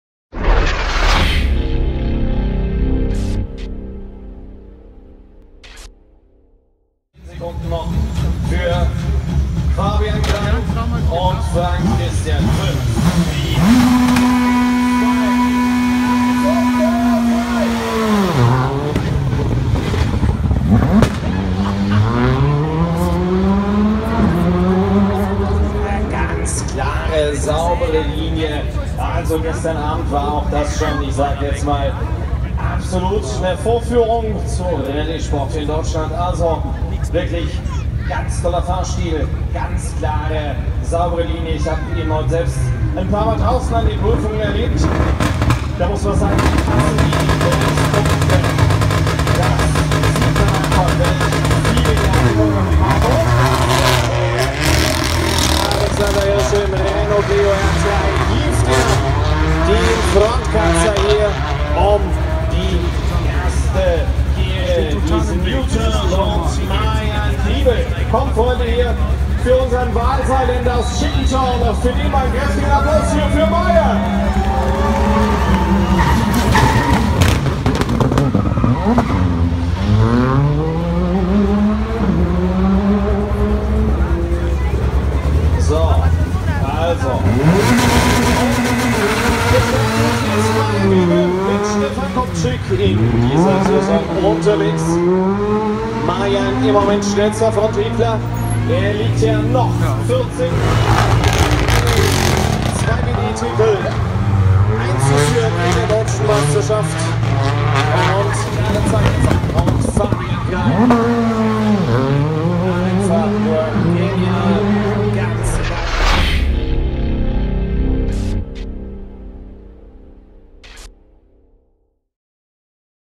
ADAC Saarland-Pfalz-Rallye 2016 | WP 13 "Action in der Innenstadt St. Wendel"
Action in der Innenstadt von St. Wendel am Freitagabend bei der WP 13.